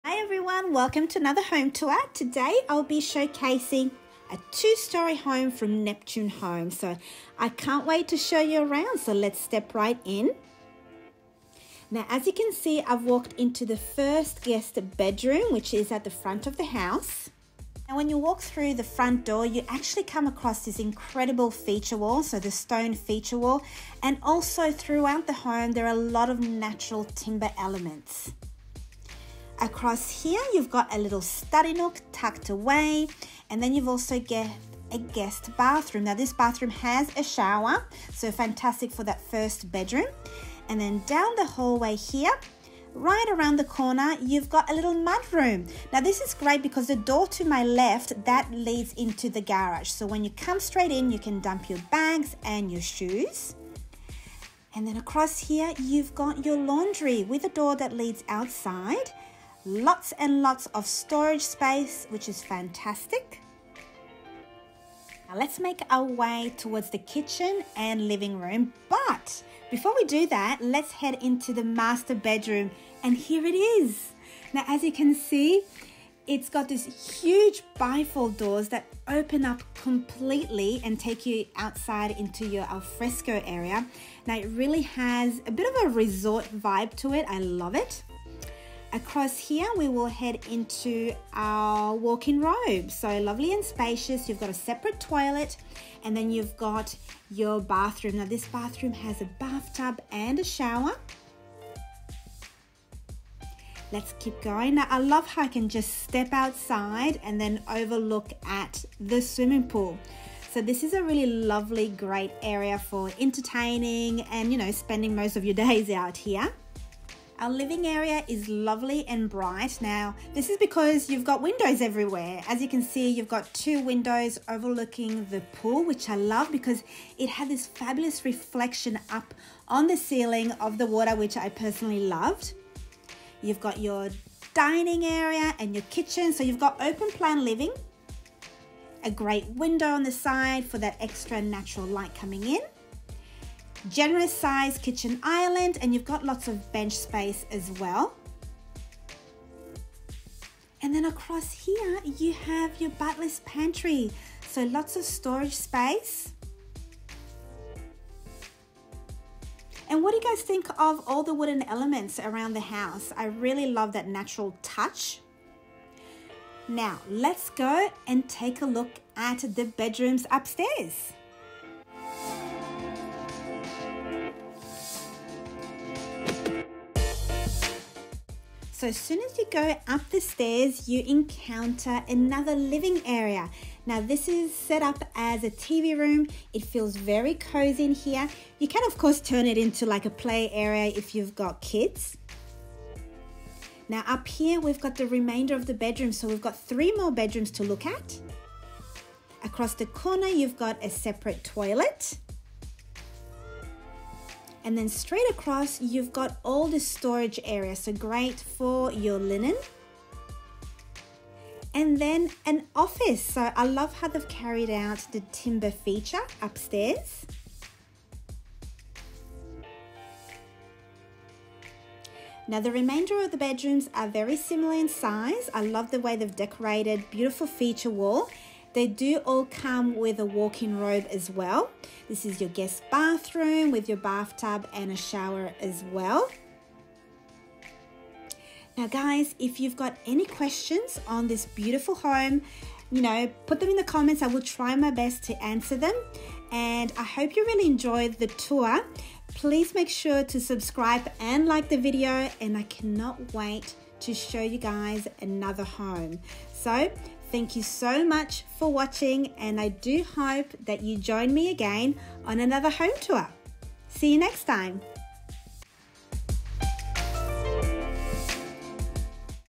Modern Resort Style Home Tour
Today, I'm taking you on a tour of the beautiful home called Hydeaway 36 by Neptune Homes in Queensland, Australia. This resort style home features five bedrooms, three baths, three living areas and two study nooks. It's the perfect place to relax and unwind, or entertain guests in style.

We'll start our tour in the large hallway, accented with timber and stonework. The entry of the home has a guest bedroom, a study nook, a powder room, and a mud room. The house flows beautifully through this area.

Next, we'll take a look at the kitchen. It's fully equipped with stainless steel appliances, granite countertops, and a breakfast bar. There's also a walk-in pantry for storing all your food and kitchenware. The pantry has a sink, dishwasher and a microwave.

The dining room is located just off the kitchen. It's a great place to gather for family meals or formal dinners. The table can seat up to 8 people, and there's plenty of space for everyone to spread out.

Connected to the dining room is a spacious living room. The high ceilings and large windows let in plenty of natural light.

The master bedroom is located on the main floor. It has its own private bathroom, walk-in closet, and direct access to the alfresco area with beautiful views of the pool. The bathroom features a soaking tub and separate shower. The walk-in closet is big enough to fit all of your clothes and accessories.

The other bedrooms are located on the second floor. Each bedroom has its own private walk-in closet.

The backyard is just as impressive as the interior of the home. There's a large swimming pool and a covered patio. There's also plenty of space for a barbecue or to just relax and enjoy the outdoors.